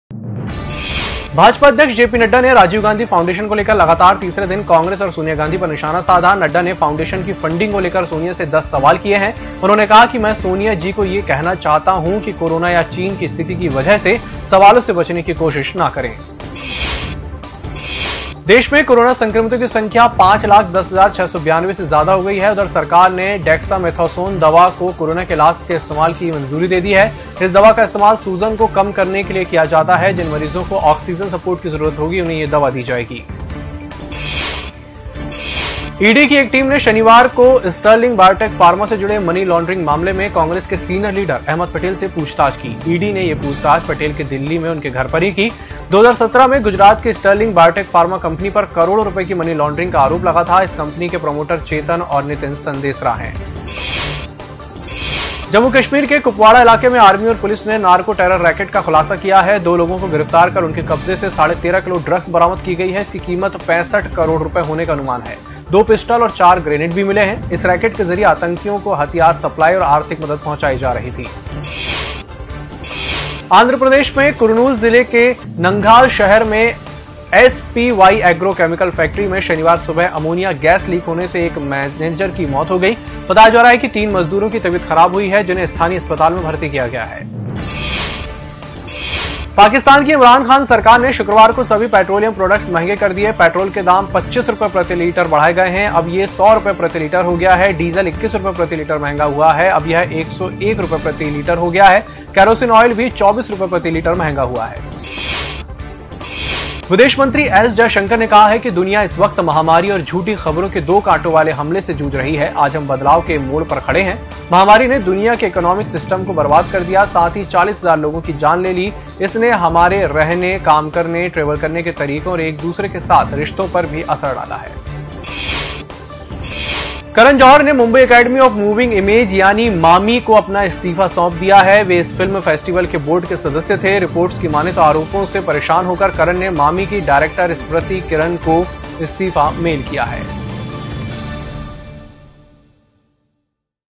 News @ 8 PM - जेपी नड्डा के सोनिया गांधी से 10 सवाल
1 जेपीनड्डा के सोनियागांधी से 10 सवाल
भाजपा अध्यक्ष जेपी नड्डा ने राजीवगांधीफाउंडेशन को लेकर लगातार तीसरे दिन कांग्रेस और सोनिया गांधी पर निशाना साधा। नड्डा ने फाउंडेशन की फंडिंग को लेकर सोनिया से 10 सवाल किए हैं। उन्होंने कहा कि मैं सोनिया जी को ये कहना चाहता हूं कि कोरोना या चीन की स्थिति की वजह से सवालों से बचने को कोशिश नहीं करें।
2 5 लाख 10 हजार से पार कोरोना संक्रमित
देश में कोरोना संक्रमितों की संख्या 5 लाख 10 हजार 692 हो गई। उधर, सरकार ने डेक्सामेथासोन दवा को कोरोना के इलाज में इस्तेमाल करने की मंजूरी दे दी है। इस दवा का इस्तेमाल सूजन को कम करने के लिए किया जाता है। जिन मरीजों को ऑक्सीजन सपोर्ट की जरूरत होगी, उन्हें यह दवा दी जाएगी।
3 अहमदपटेल के घर पहुंची ईडी की टीम
ईडी की एक टीम ने शनिवार को संदेसरा बंधुओं (स्टर्लिंग बायोटेक फार्मा) से जुड़े मनी लॉन्ड्रिंग मामले में कांग्रेस के सीनियर लीडर अहमद पटेल से पूछताछ की। ईडी ने यह पूछताछ पटेल के दिल्ली में उनके घर की। 2017 में गुजरात की स्टर्लिंग बायोटेक फार्मा कंपनी पर करोड़ों रुपए की मनी लॉन्ड्रिंग का आरोप लगा था। इस कंपनी के प्रमोटर चेतन और नितिन संदेसरा हैं।
4 जम्मू-कश्मीर में नार्को-टेरर रैकेट
जम्मू-कश्मीर के कुपवाड़ा इलाके में आर्मी और पुलिस ने नार्को-टेरर रैकेट का खुलासा किया है। 2 लोगों को गिरफ्तार कर उनके कब्जे से 13.5 किलो ड्रग्स बरामद की। इसकी कीमत 65 करोड़ रुपए होने का अनुमान है। 2 पिस्टल और 4 ग्रेनेड भी मिले हैं। इस रैकेट के जरिए आतंकियों को हथियार सप्लाई और आर्थिक मदद पहुंचाई जा रही थी।
5 आंध्रप्रदेश में फैक्ट्री से गैसलीक हुई
आंध्रप्रदेश में कुर्नूल जिले के नंद्याल शहर में एसपीवाई एग्रो केमिकल फैक्ट्री में शनिवार सुबह अमोनिया गैस लीक होने से एक मैनेजर की मौत हो गई। बताया जा रहा है कि तीन मजदूरों की तबीयत खराब हुई है, जिन्हें स्थानीय अस्पताल में भर्ती किया गया है।
6 पाकिस्तान में पेट्रोल-डीजल और महंगा
पाकिस्तान की इमरानखान सरकार ने शुक्रवार को सभी पेट्रोलियम प्रोडक्ट्स महंगे कर दिए। पेट्रोल के दाम 25.58 रुपए प्रति लीटर (पाकिस्तान करंसी में) बढ़ाए गए हैं। अब यह 100.10 रुपए प्रति लीटर हो गया है। डीजल 21 रुपए प्रति लीटर महंगा हुआ है। अब यह 101.46 रुपए प्रति लीटर हो गया है। केरोसिन ऑयल भी 24 रुपए प्रति लीटर महंगा हुआ है।
7 दुनिया महामारी और झूठी खबरों से जूझ रही - विदेश मंत्री
विदेशमंत्री एस.जयशंकर ने कहा है कि दुनिया इस वक्त महामारी और झूठी खबरों के दो कांटों वाले हमले से जूझ रही है। आज हम बदलाव के मोड़ पर खड़े हैं। महामारी ने दुनिया के इकोनॉमिक सिस्टम को बर्बाद कर दिया, साथ ही 40 हजार लोगों की जान ले ली। इसने हमारे रहने, काम करने, ट्रैवल करने के तरीकों और एक-दूसरे के साथ रिश्तों पर भी असर डाला है।
8 करन जौहर का मामीफिल्मफेस्टिवल के बोर्ड से इस्तीफा
करनजौहर ने मुंबईएकेडमीऑफमूविंगइमेज यानी मामी को अपना इस्तीफा सौंप दिया है। वे इस फिल्म फेस्टिवल के बोर्ड के सदस्य थे। रिपोर्ट्स की मानें तो आरोपों से परेशान होकर करन ने मामी की डायरेक्टर स्मृति किरण को इस्तीफा मेल किया है।